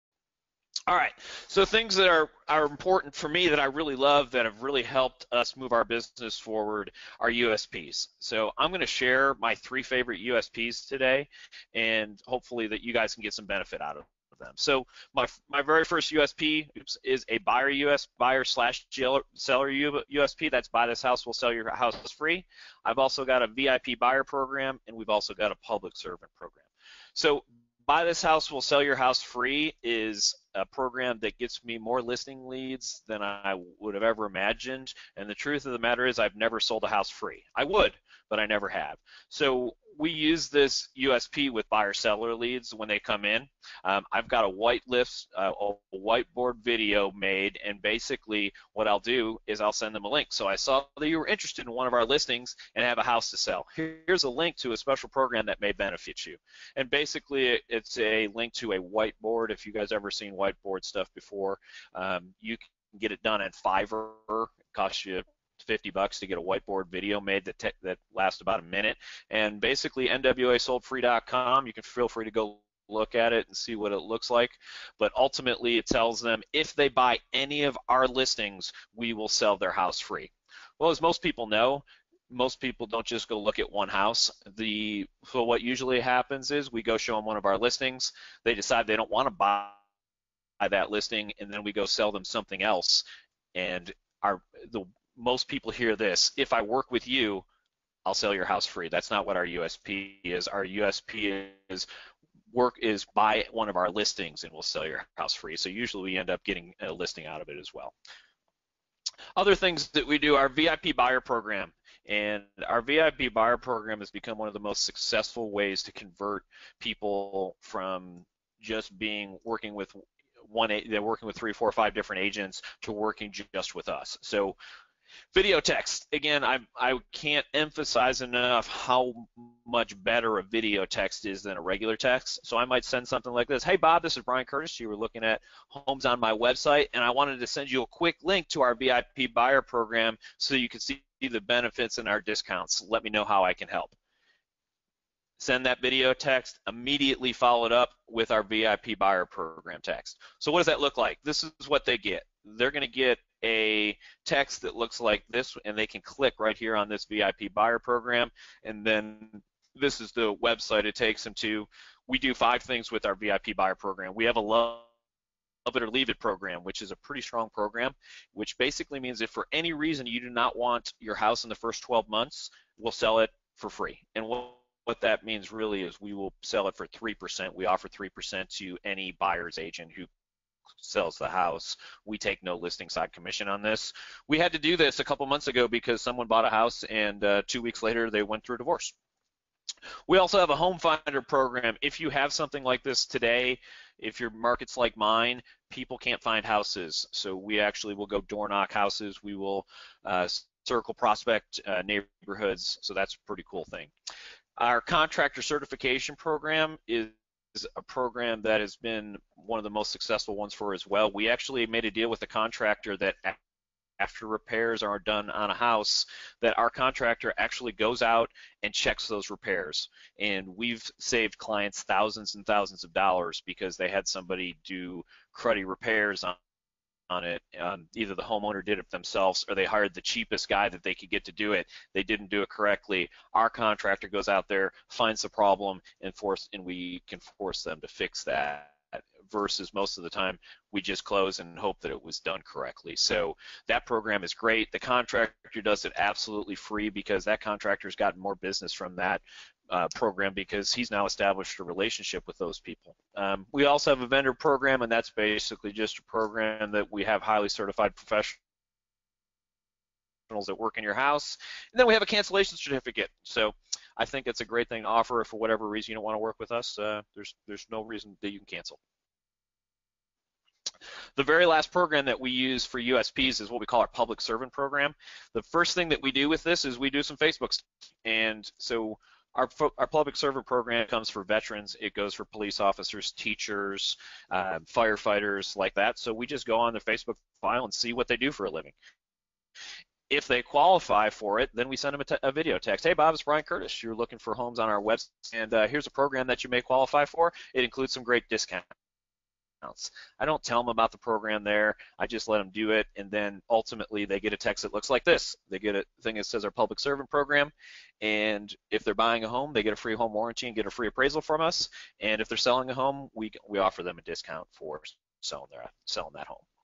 Key USP's (Unique Sales Propositions)
1 - Buy this house we will sell your house FREE
2 - VIP Buyer Program
3 - Public Servant Program